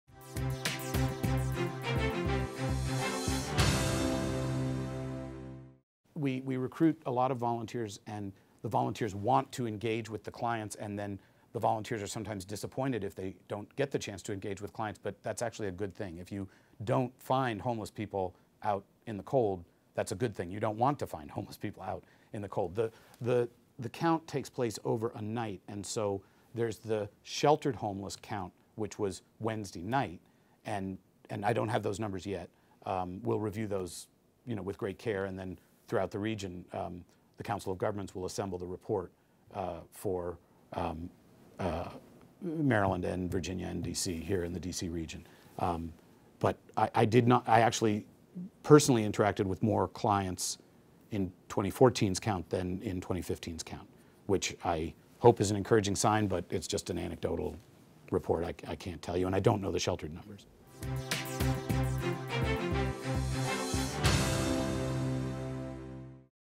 Council President George Leventhal Recaps Recent Homeless Count
We are still waiting for results from the overnight homeless count that recently took place in Montgomery County.

In this MyMCMedia Extra video, Montgomery County Council President George Leventhal says he saw fewer homeless people this year than last but he’s says it’s too early to know if the numbers are officially down or not. Take a look: